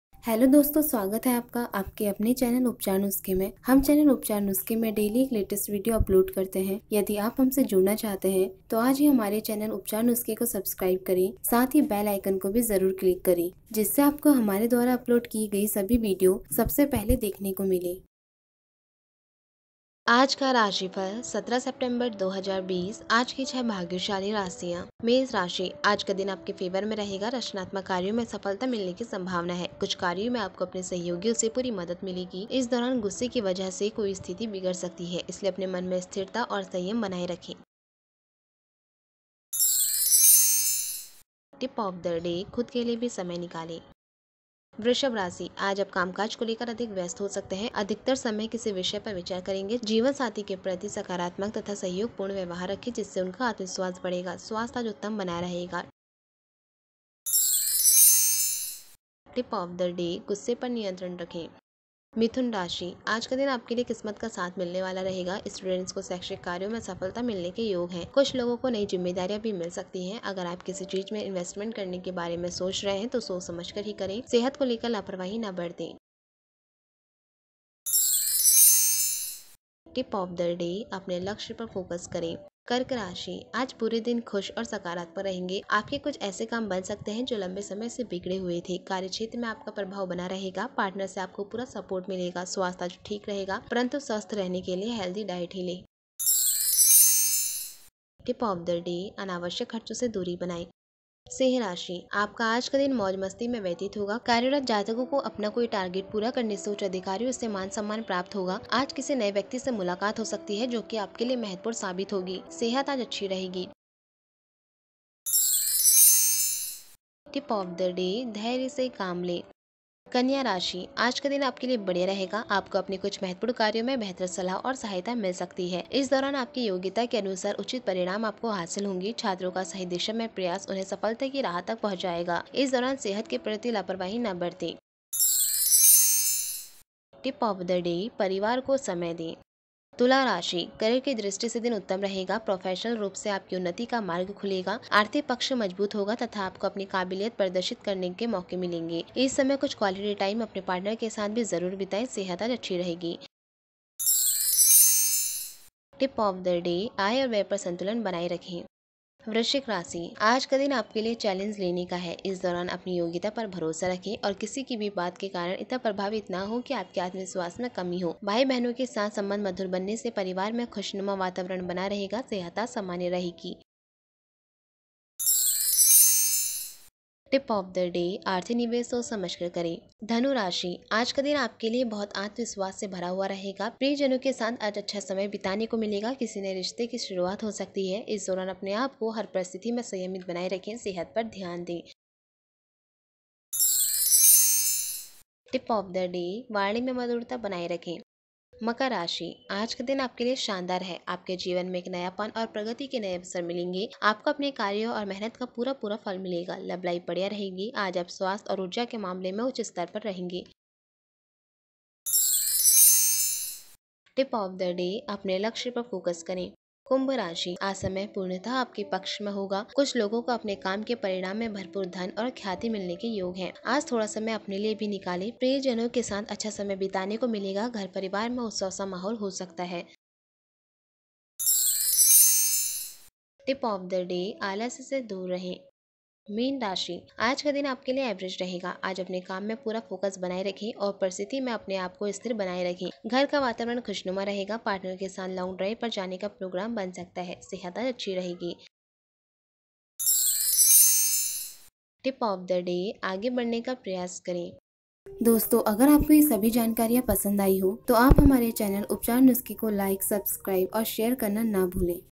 Aaj ka Rashifal in Hindi Today Horoscope Daily राशिफल 17 सितम्बर 2020
आज हम आपको 17 सितम्बर 2020 का राशिफल हिंदी में बताने जा रहे है यहां पर आप सभी 12 राशियों का दैनिक राशिफल जान सकते है. तो आईये जानते है सभी 12 राशियों के लिए 17 सितम्बर 2020 आज का राशिफल (17 September ka Rashifal) कैसा रहेगा.